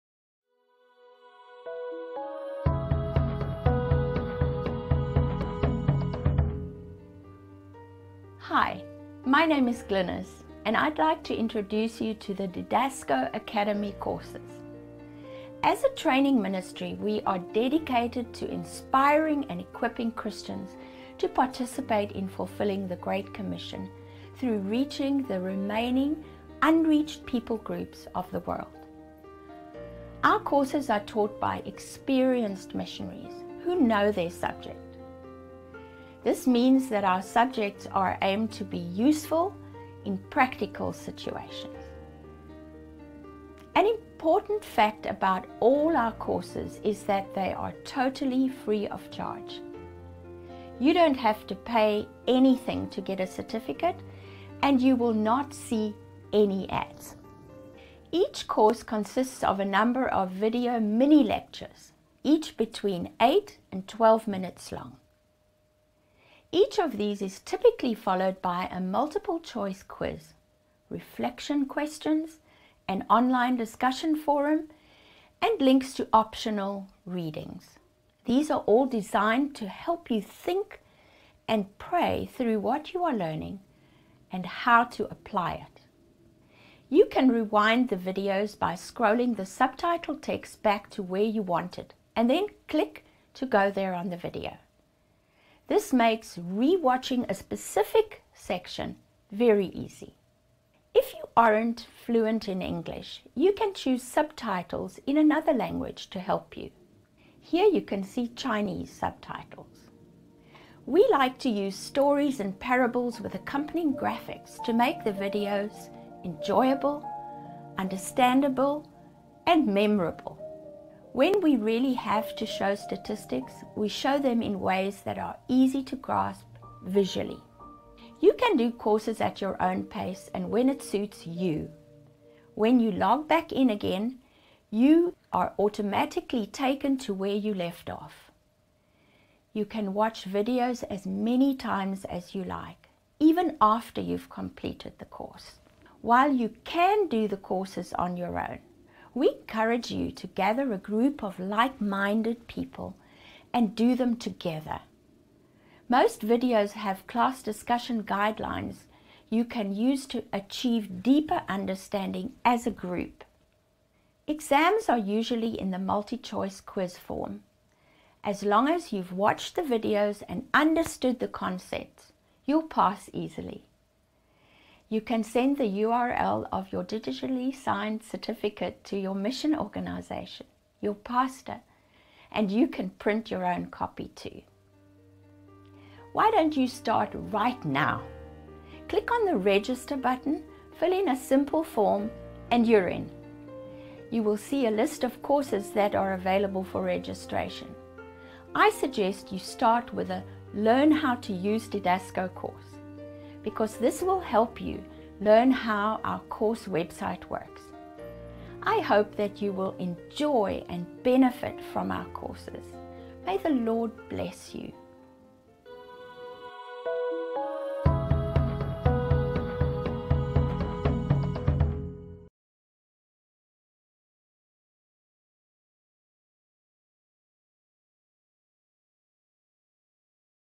Get to know Didasko Missions Academy
A quick overview of how Didasko Missions Academy courses work.